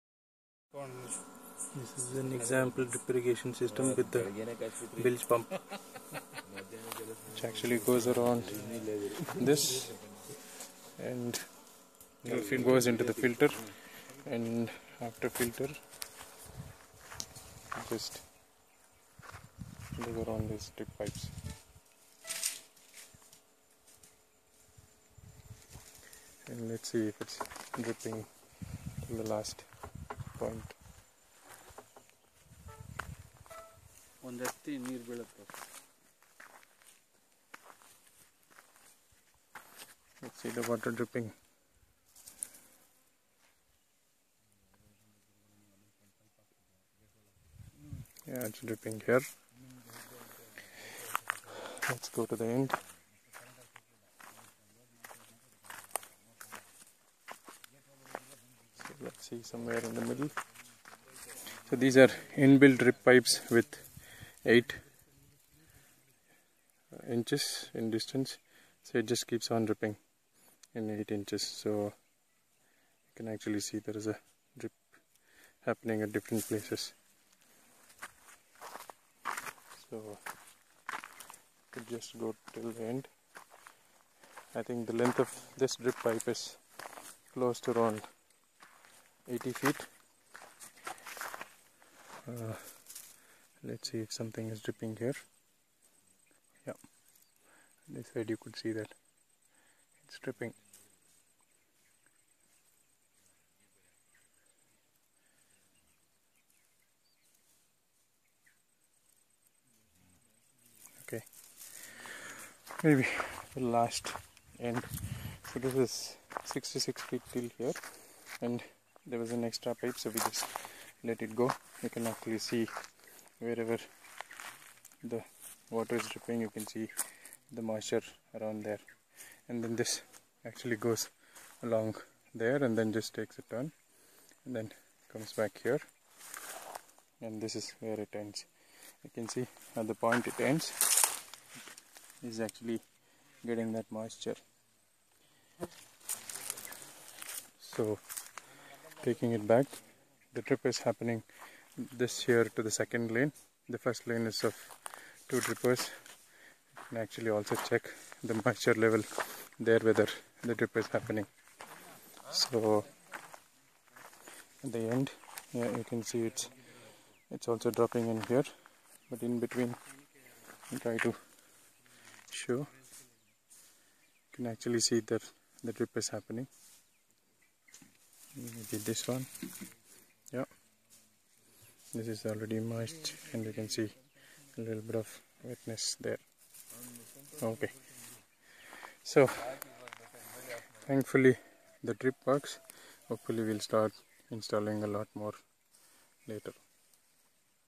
Drip Irrigation using a Bilge Pump (Capacity 2000GPH)
Here's an experiment that we trying to use some sort of DC pump for drip irrigation. Since we were told drip by gravity doesn't work, we tried to use these cheaper Bilge Pumps for drip irrigation for our small garden area. Finally here's the result.